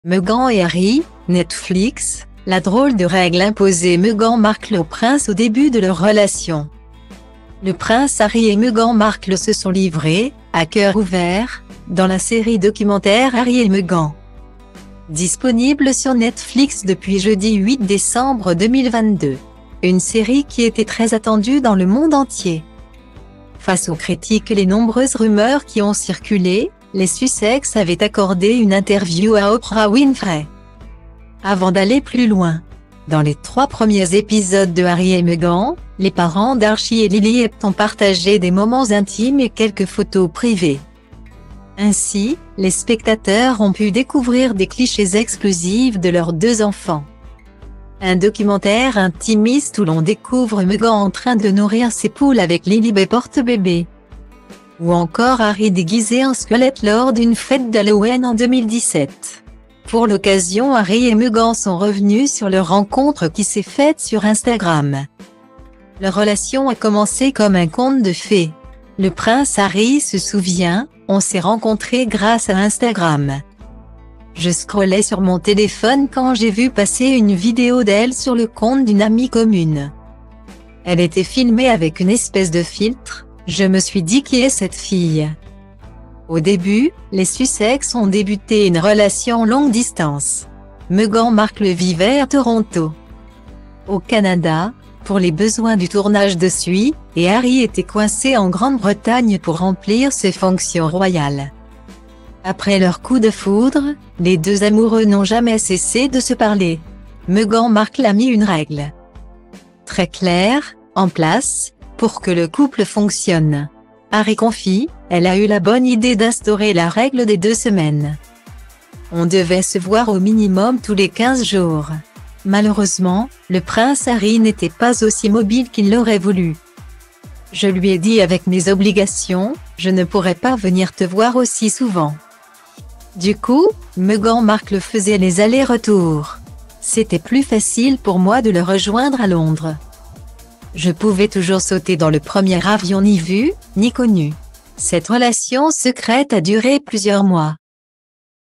Meghan et Harry (Netflix) : la drôle de règle imposée Meghan Markle au prince au début de leur rela
Meghan et Harry (Netflix) : la drôle de règle imposée  Meghan Markle au prince au début de leur relation
Le prince Harry et Meghan Markle se sont livrés, à cœur ouvert, dans la série documentaire Harry et Meghan,disponible sur Netflix depuis jeudi 8 décembre 2022. Une série qui était très attendue dans le monde entier.Face aux critiques et les nombreuses rumeurs qui ont circulé, les Sussex avaient accordé une interview à Oprah Winfrey.